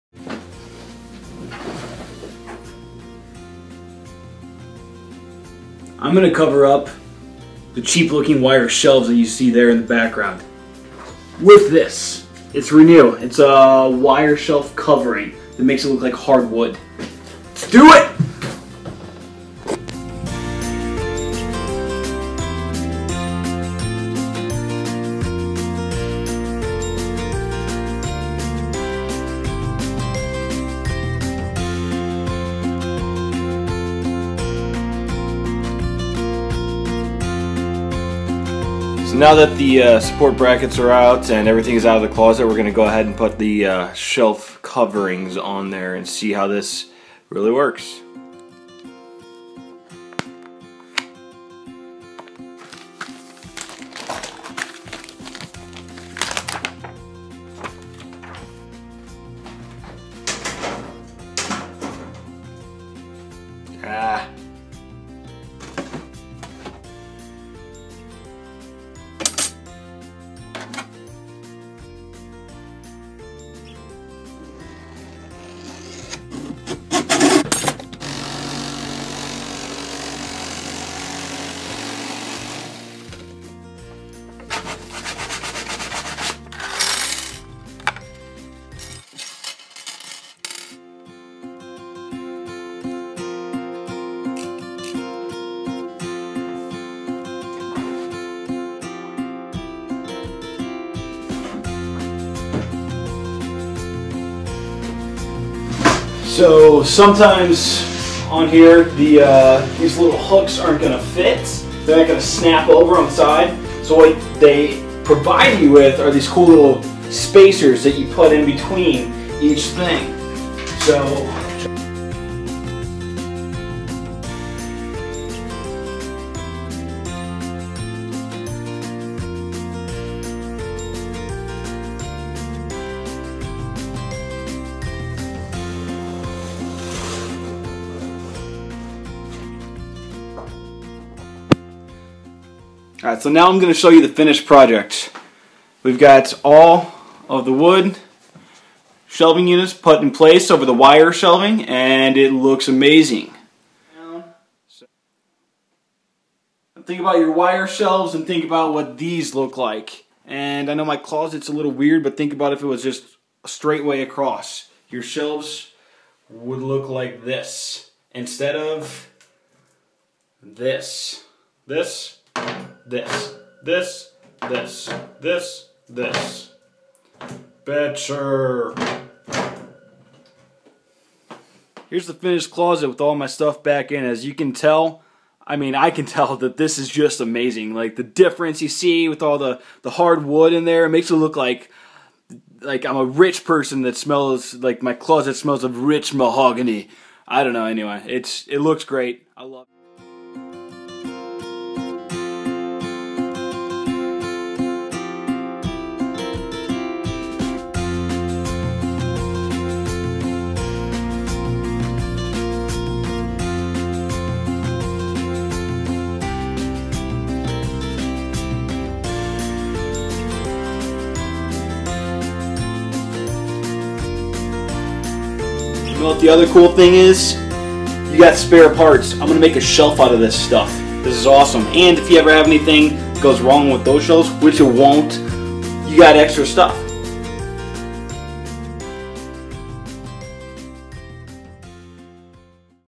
Redesign of my Closet with Renew (wire shelf coverings)!
Re-designed my Closet. It looks so much better now. I used a wood shelf covering and draped over my cheap looking wire shelves in the closet. The difference was influential. Now I keep my closet more organized, press my clothes more often and delight in walking into the small space. This product is called Renew and it's from John Louis Home. it's durable and easy and I love it.